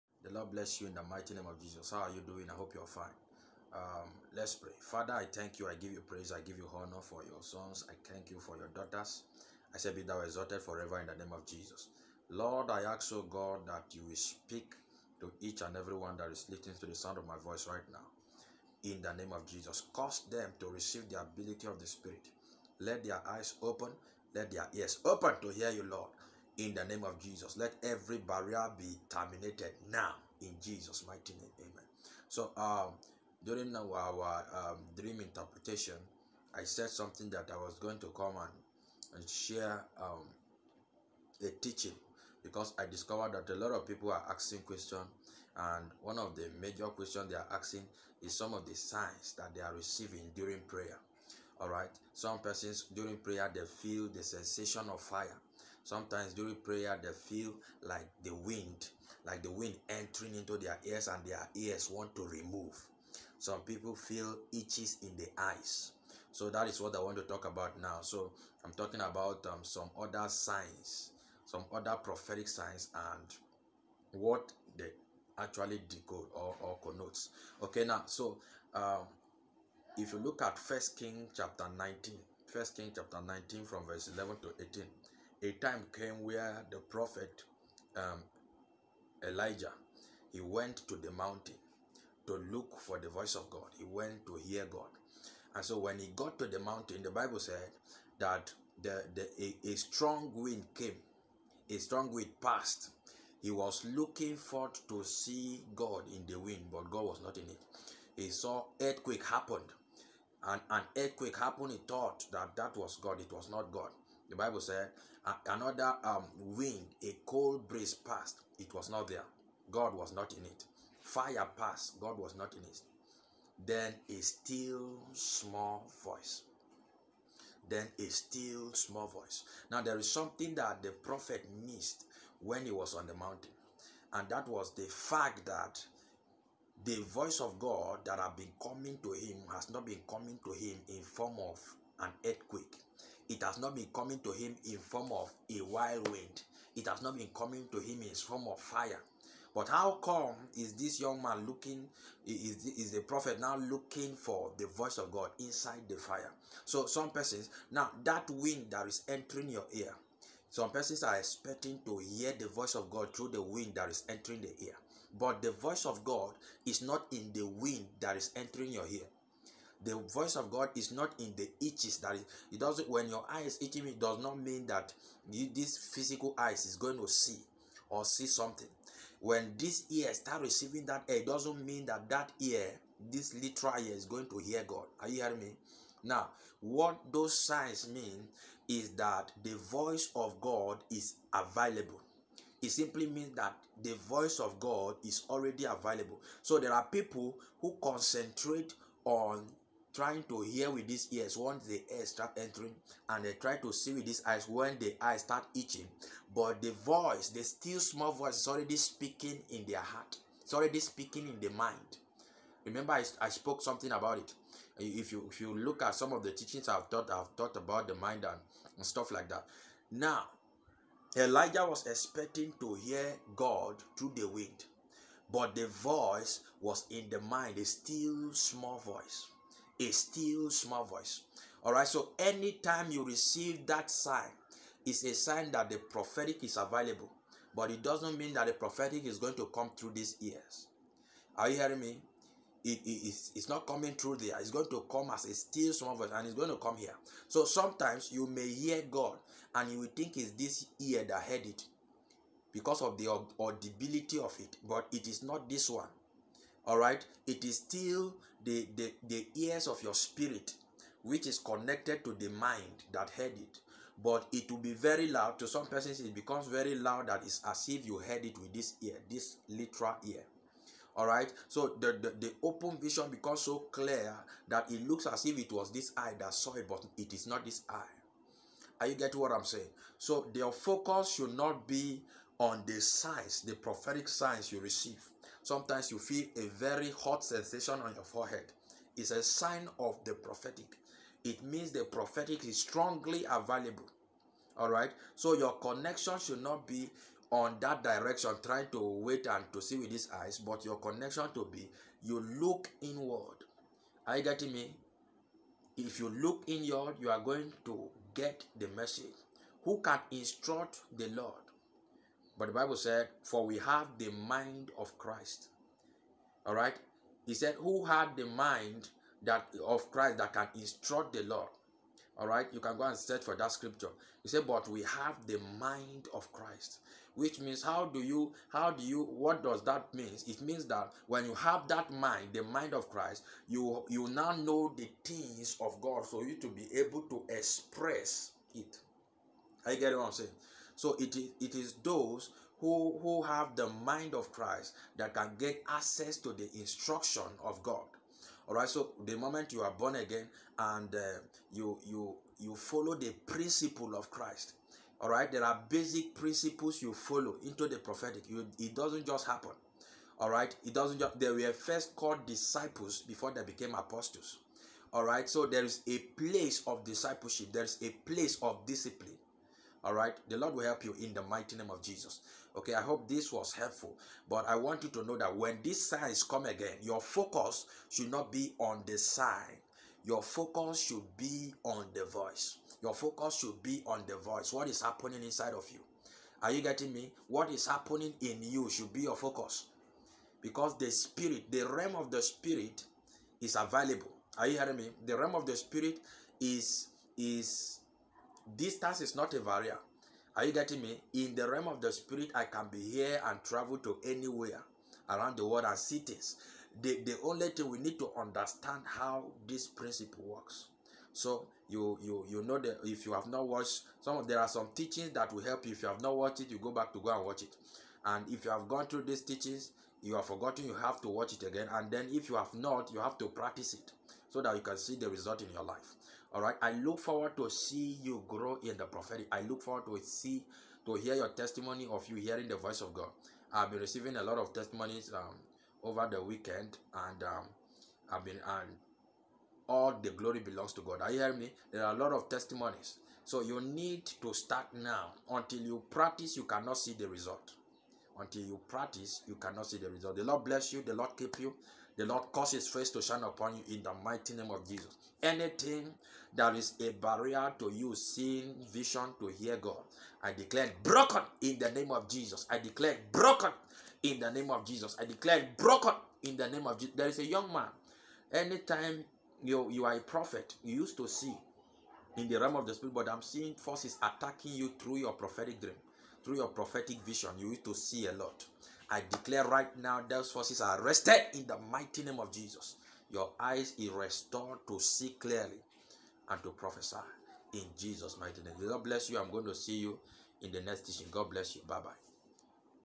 Prophetic Signs and meaning part 2
most signs that we experience during spiritual exercises has meaning, some are messages divinity is passing directly to us, while some are signs of an available message.

ask your questions, drop your comments

if you are new, watch from the beginning to get details of the prophetic. God bless you